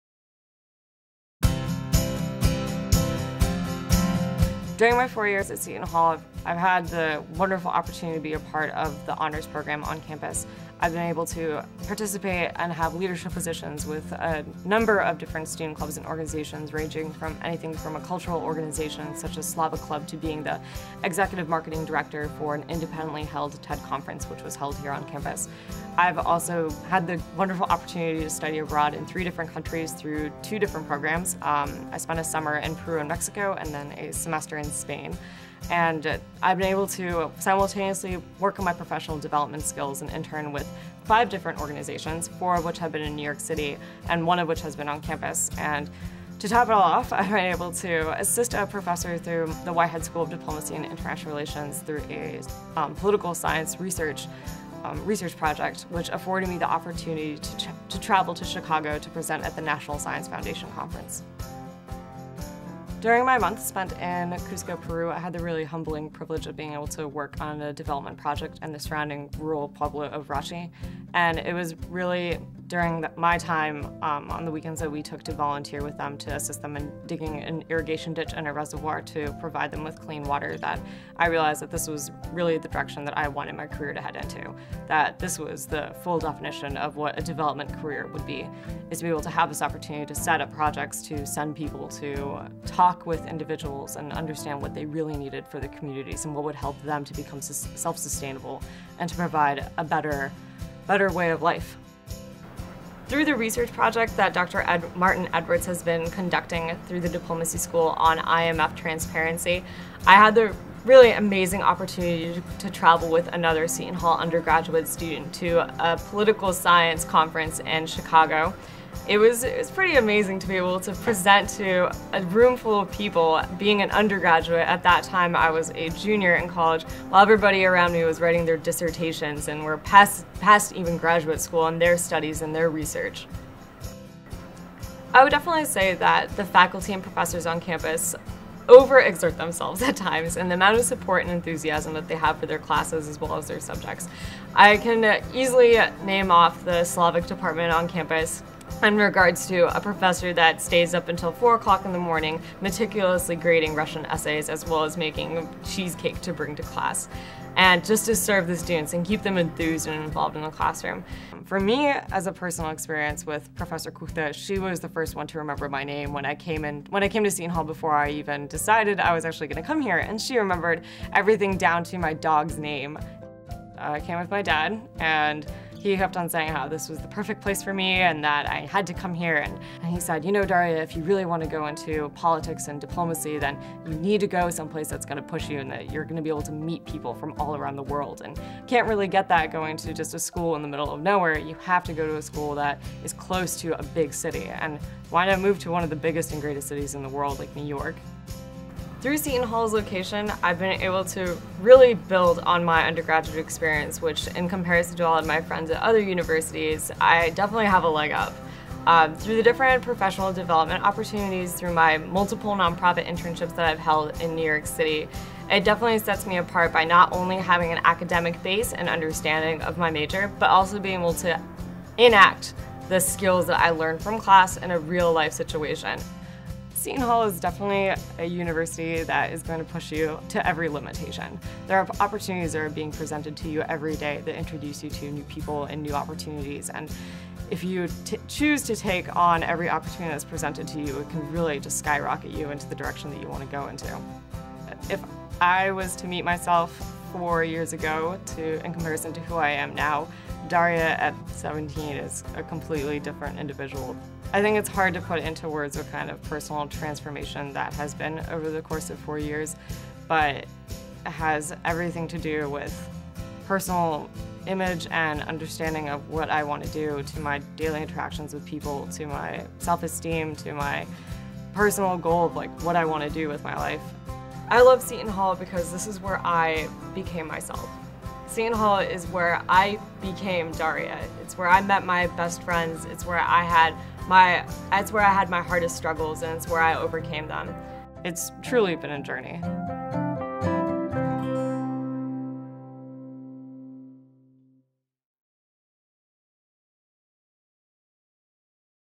Meet Our Leaders: Daria Preston *
Daria Preston, a Diplomacy and International Relations and Modern Languages Major and Economics Minor from Palmer, Alaska